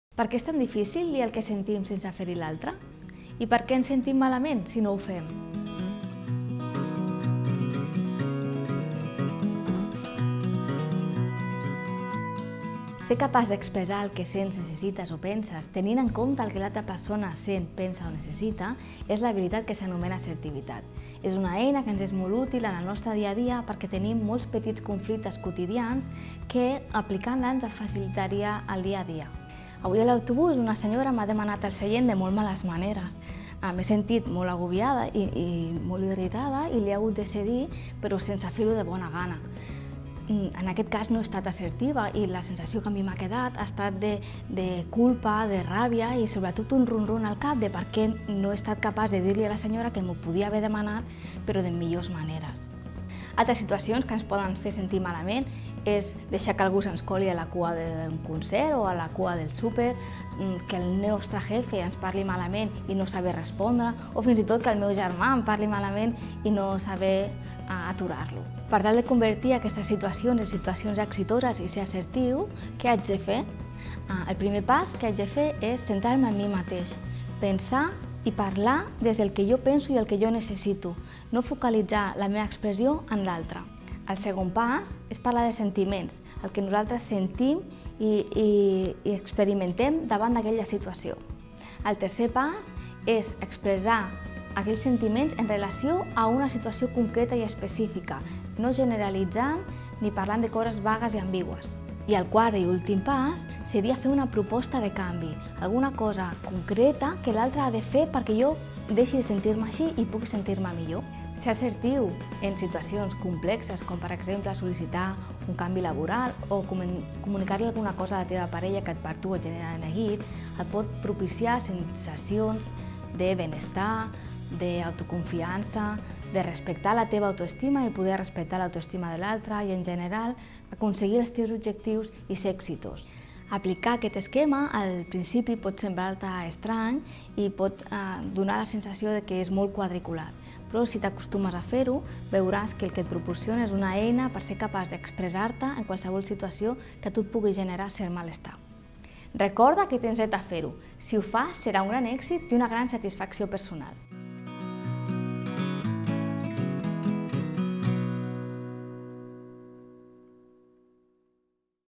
Comunicar-se assertivament
Ser capaç d’expressar el que sents, necessites o penses tenint en compte el que l’altre persona sent, pensa o necessita és l’habilitat que s’anomena assertivitat. La Lisette Navarro, professora de la Facultat d'Educació Social i Treball Social Pere Tarrés - URL, ens explica com aquesta eina ens pot ser molt útil en el nostre dia a dia per resoldre molts petits conflictes quotidians.
🔸 Pots llegir "Perquè és tan difícil dir el que sentim sense ferir a l’altre?"